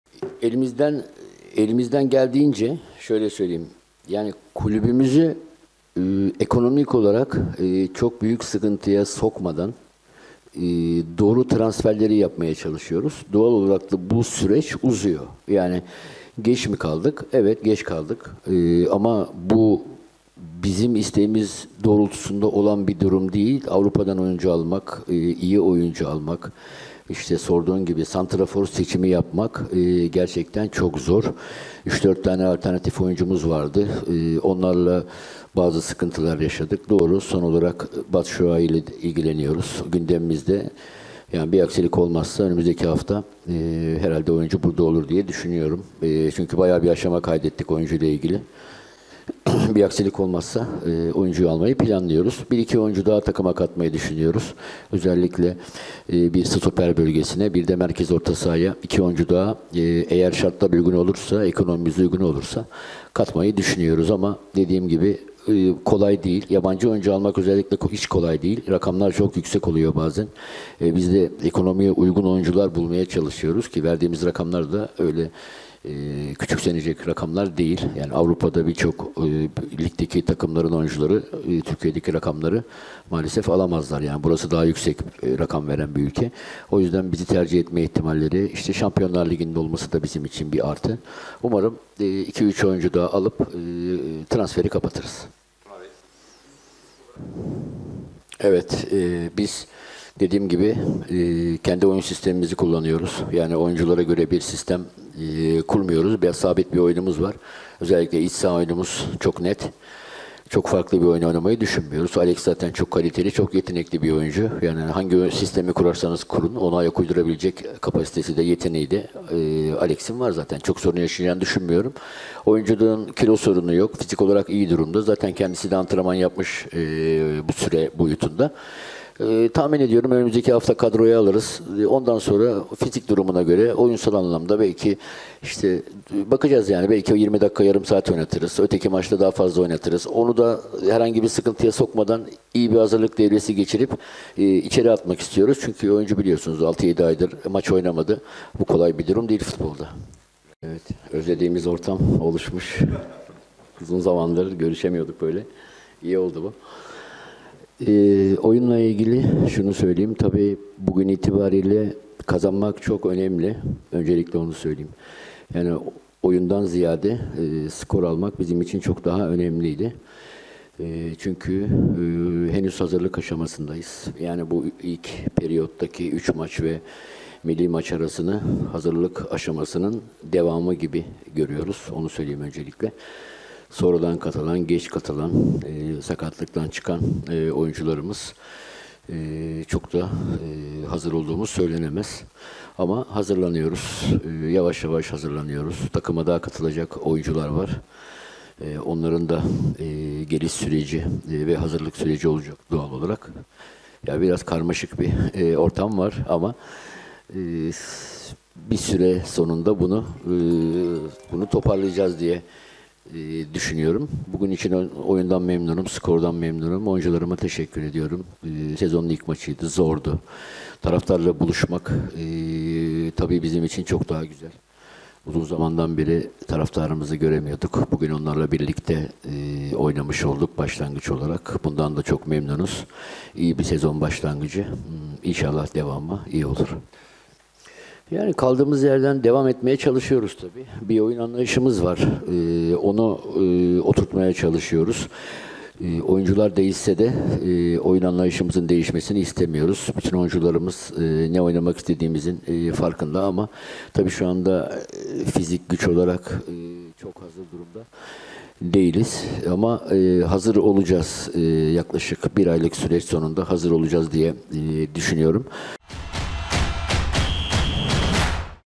Sergen Yalçın golcü transferini resmen açıkladı! "Batshuayi önümüzdeki hafta burada olacak"
Beşiktaş Teknik Direktörü Sergen Yalçın, Rizespor maçının ardından Batshuayi transferinde önemli mesafe aldıklarını ve bir aksilik olmazsa oyuncunun haftaya İstanbul'da olacağını söyledi.